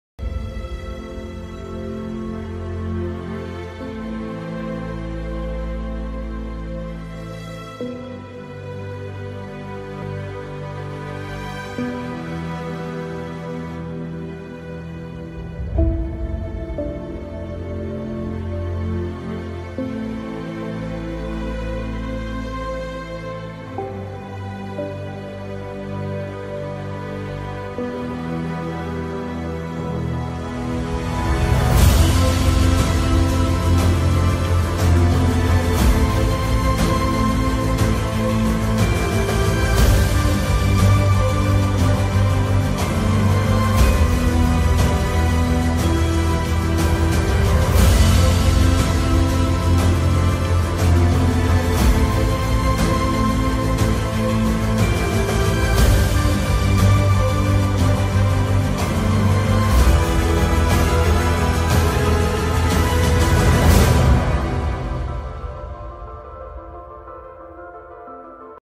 Trailer dự án căn hộ La Partenza Nam Sài gòn quận 7
Dự án Căn hộ La Partenza Nam Sài Gòn Quận 7 là chung cư cao cấp tiếp theo được Chủ đầu tư Khải Minh Land ra mắt tại Thành phố Hồ Chí Minh, Dự án La Partenza được lấy cảm hứng từ lối sống thượng lưu của công quốc Monaco, kết hợp cùng nhiều tính năng nổi bật giúp mang đến sự sang trọng đẳng cấp cho cư dân nơi đây.

Vị trí dự án La Partenza bao bồm 3 Block căn hộ, lên tới 1245 căn hộ chung cư cao cấp và 27 Shophouse thương tại tầng trệt...